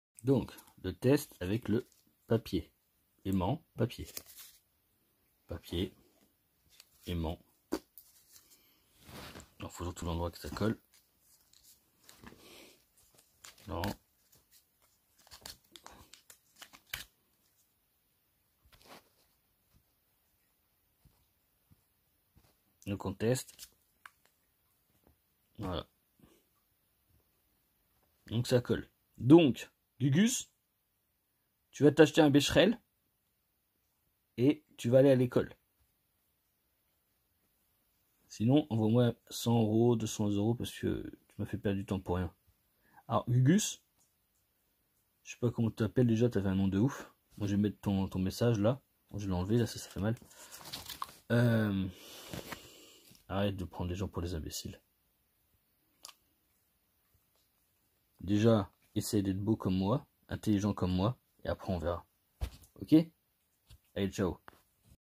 Nouveau Test, un aimant colle à ma peau alors que je mets un papier sur moi pour réduire son champ.
Après ma discussion avec Guguss, sur Facebook,  qui a des problèmes d'écriture, de lecture et de compréhension,  jai suivi sa demande par pitié :

- Torse vertical,
- Papier entre un aimant et ma peau.